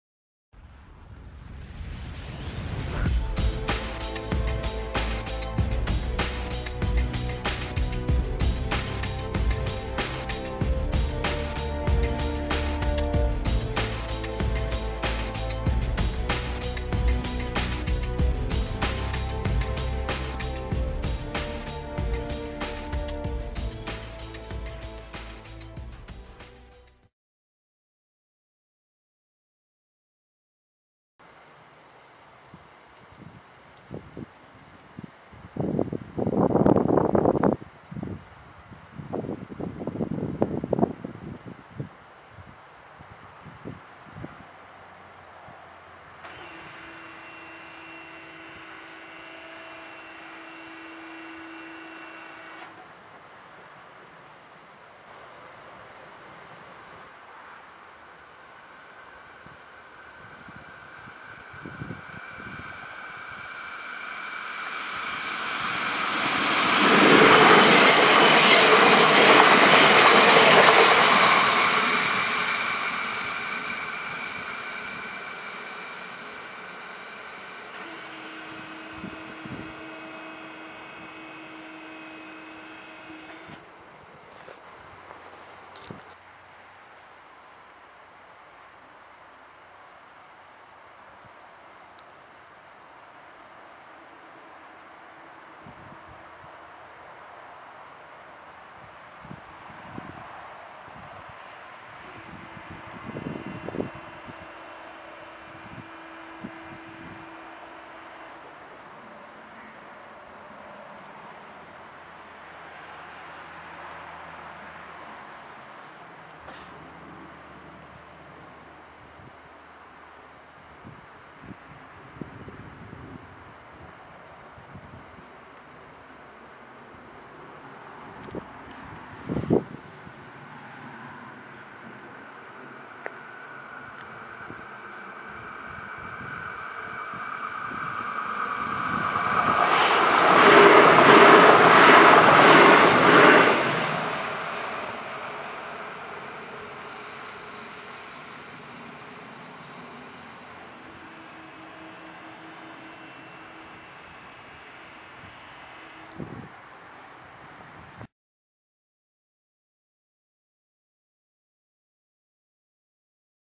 Bahnübergang Aufhausen (Württ.) "Feldweg"
Aufhausen liegt unweit von Bopfingen und an der Riesbahn. An einem Wirtschaftweg oberhalb des Ortes liegt dieser Bahnübergang. Obwohl sich der Verkehr hier zum größten Teil auf die Landwirtschaft und Fahrradfahrer beschränkt, hat man neben den Ampeloptiken von Pintsch Bamag auch eine Halbschrankenanlage installiert. Zu sehen gibt es hier die Baureihe 440, mit der größte Teil des Verkehres auf der Riesbahn durchgeführt wird.